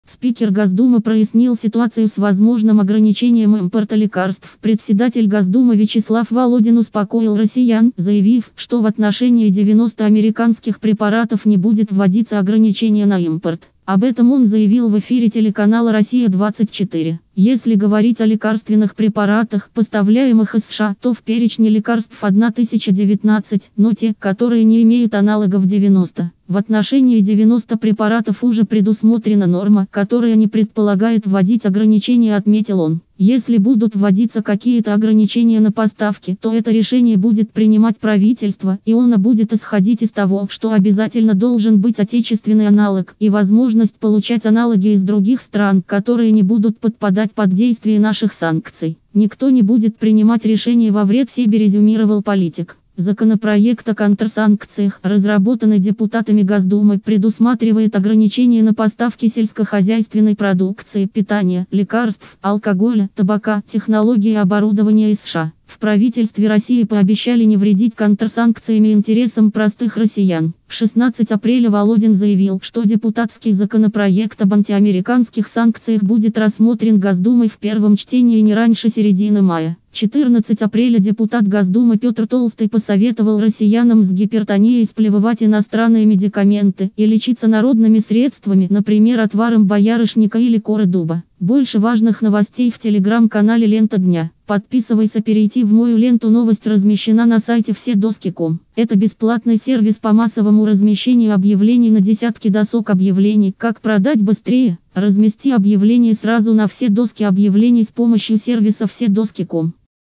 Спикер Госдумы прояснил ситуацию с возможным ограничением импорта лекарств.
Председатель Госдумы Вячеслав Володин успокоил россиян, заявив, что в отношении 90 американских препаратов не будет вводиться ограничения на импорт. Об этом он заявил в эфире телеканала «Россия 24».

«Если говорить о лекарственных препаратах, поставляемых из США, то в перечне лекарств 1019, но те, которые не имеют аналогов — 90. В отношении 90 препаратов уже предусмотрена норма, которая не предполагает вводить ограничения», — отметил он.

«Если будут вводиться какие-то ограничения на поставки, то это решение будет принимать правительство, и оно будет исходить из того, что обязательно должен быть отечественный аналог и возможность получать аналоги из других стран, которые не будут подпадать под действие наших санкций. Никто не будет принимать решение во вред себе», — резюмировал политик.

Законопроект о контрсанкциях, разработанный депутатами Госдумы, предусматривает ограничение на поставки сельскохозяйственной продукции, питания, лекарств, алкоголя, табака, технологий и оборудования из США. В правительстве России пообещали не вредить контрсанкциями интересам простых россиян.

16 апреля Володин заявил, что депутатский законопроект об антиамериканских санкциях будет рассмотрен Госдумой в первом чтении не раньше середины мая.

14 апреля депутат Госдумы Петр Толстой посоветовал россиянам с гипертонией «сплевывать» иностранные медикаменты и лечиться народными средствами, например отваром боярышника или коры дуба.